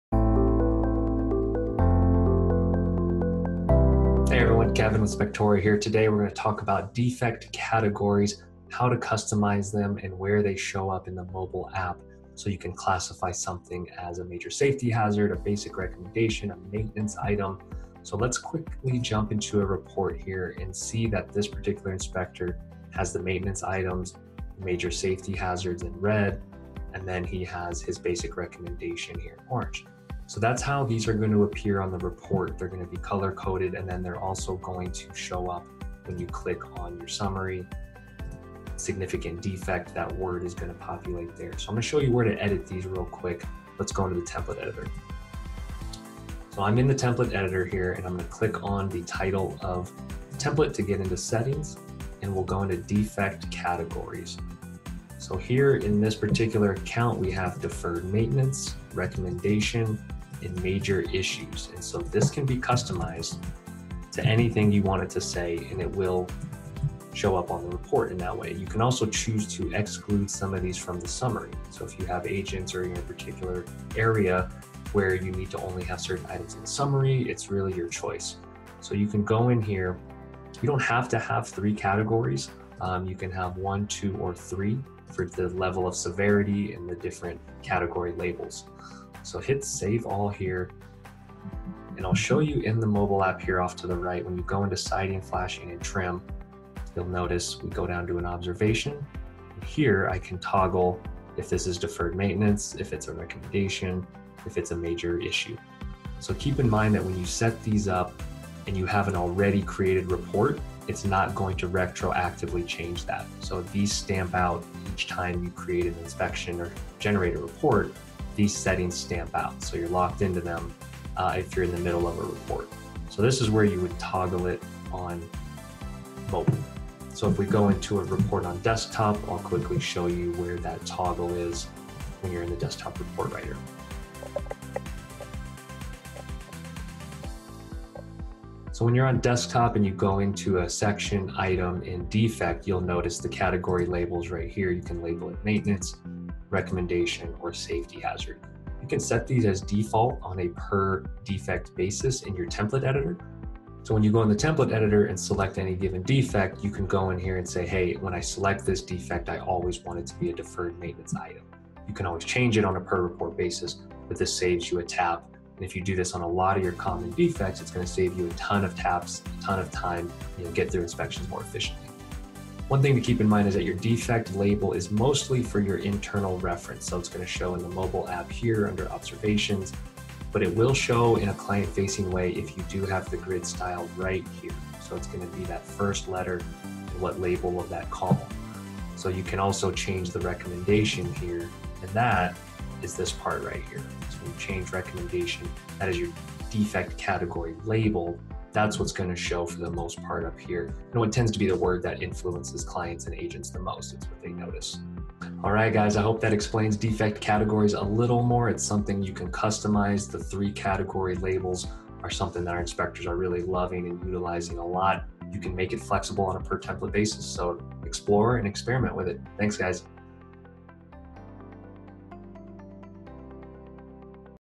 How to Customize Defect Category Labels on Spectora
Learn how to customize how you refer to your defect categories in Spectora with our custom labels. See how you can change safety hazard or recommendation on a per-template basis!